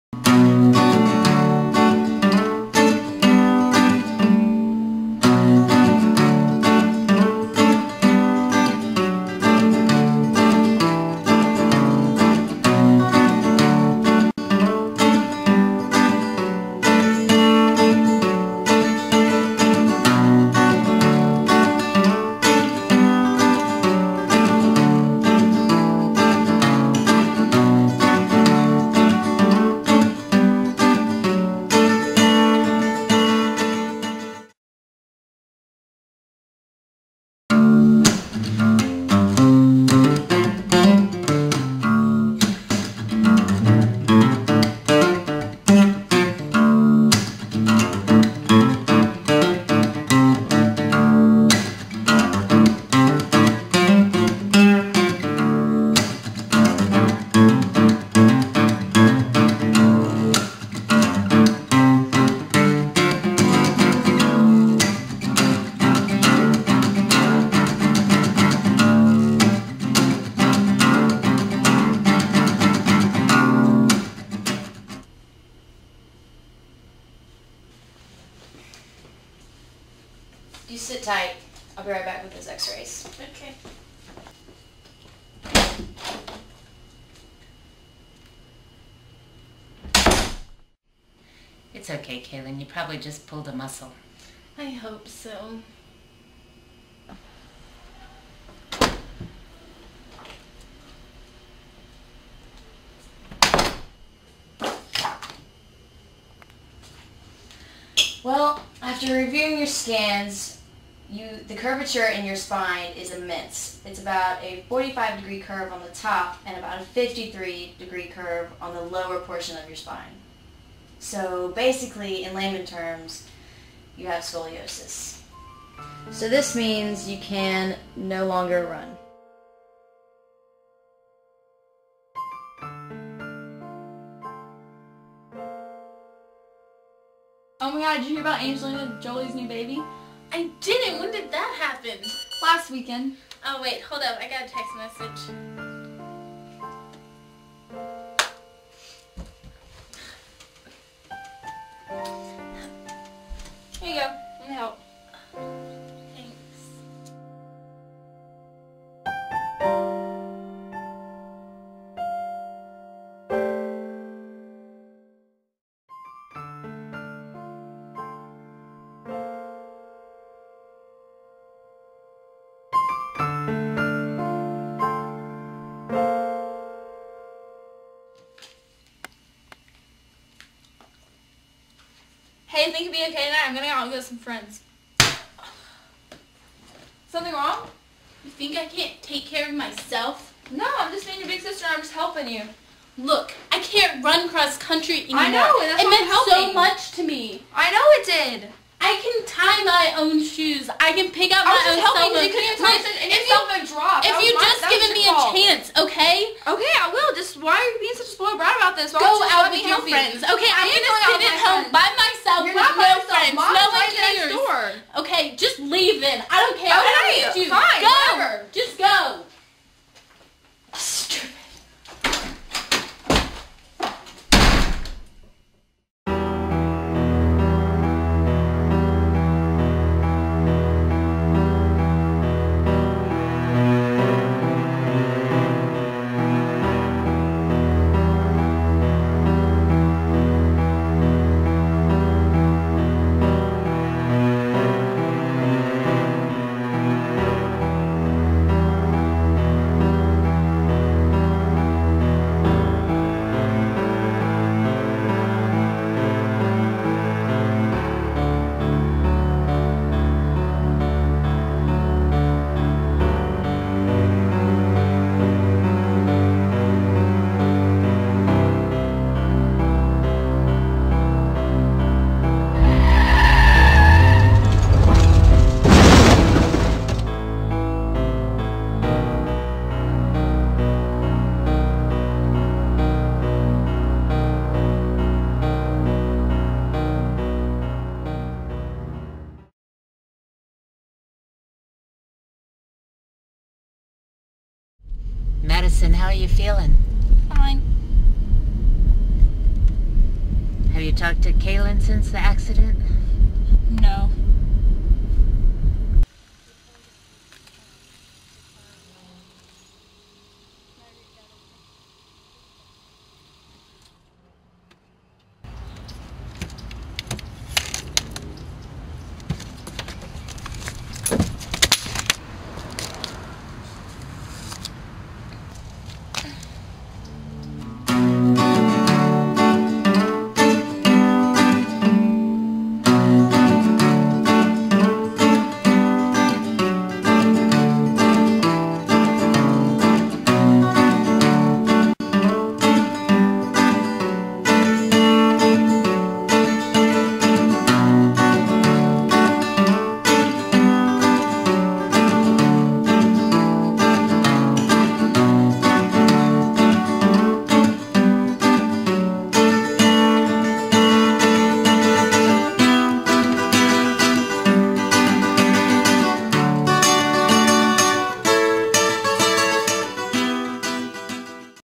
A Curve in the Road
The second film we made for the class that I am taking called Film Study IB.

The only requirements, besides the normal IB requirements, were that it had to be between 5 and 7 minutes in length.

I was the cinematographer and am being graded on the cinematography of the film.